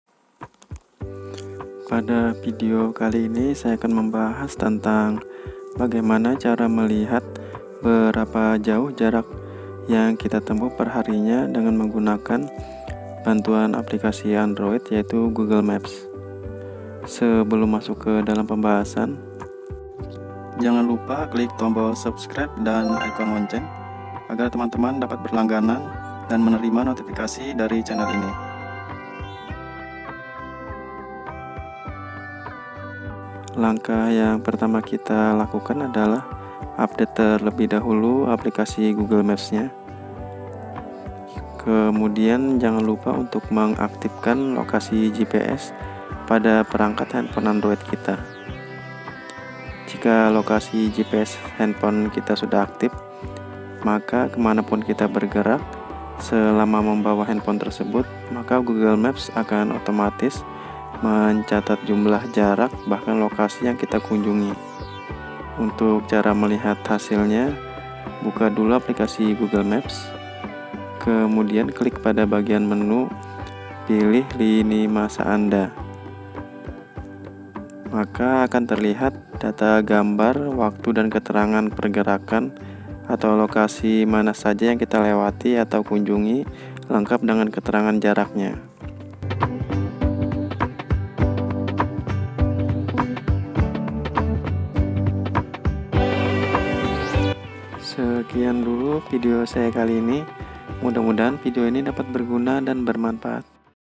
Cara melihat history/mengukur jarak yang sudah ditempuh lewat google maps
Berikut ini cara melihat history/mengukur jarak (km) yang sudah di tempuh perhari menggunakan Google maps. Langkah yang pertama kita lakukan adalah update terlebih dahulu aplikasi google maps diplaystore. Kemudian jangan lupa untuk mengaktifkan lokasi GPS pada perangkat handphone android kita. Jika lokasi GPS handphone kita sudah aktif,maka kemanapun kita bergerak selama masih membawa handphone tersebut maka google maps akan otomatis mencatat jumlah jarak bahkan lokasi yang kita kunjungi. Untuk cara melihatnya,buka terlebih dahulu aplikasi google maps kemudian klik pada bagian menu dan pilih "Linimasa anda". Maka akan terlihat detail data berupa gambar,waktu dan keterangan dari pergerakan/lokasi mana saja yang telah kita lewati/kunjungi lengkap dengan keterangan jaraknya.
Untuk lebih jelasnya,silahkan simak video lengkapnya...